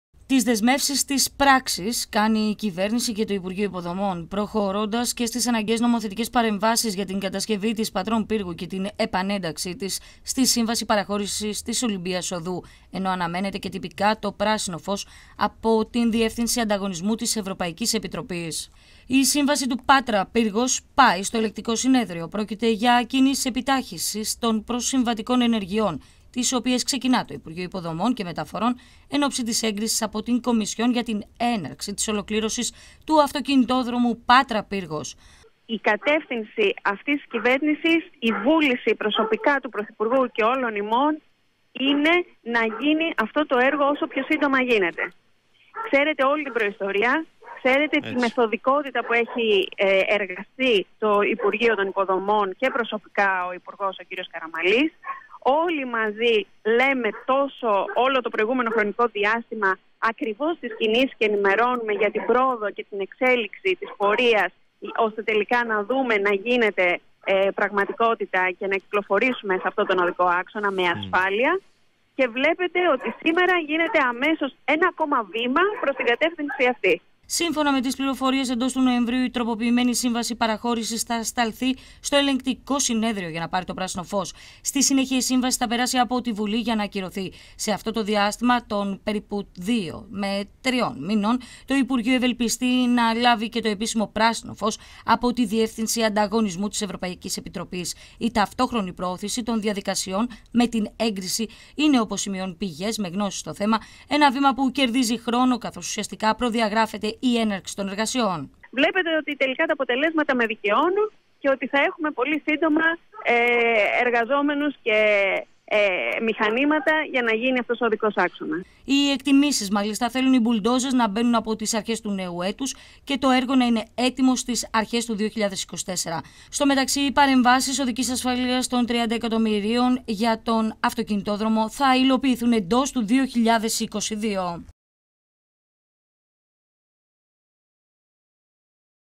Στη Βουλή η Πατρών-Πύργου
Το «πράσινο φως» από την ολομέλεια της Τετάρτης, 3 Νοεμβρίου 2021, αναμένεται να εξασφαλίσει η τροπολογία του υπουργείου Υποδομών & Μεταφορών για τη  σύμβαση παραχώρησης και τη συνέχιση των έργων στον αυτοκινητόδρομο Πάτρας-Πύργος-Τσακώνα. Γίνεται ακόμη ένα βήμα για τον σύγχρονο αυτοκινητόδρομο ενώ αναμένεται η τυπική έγκριση από τη Διεύθυνση Ανταγωνισμού της Ε.Ε.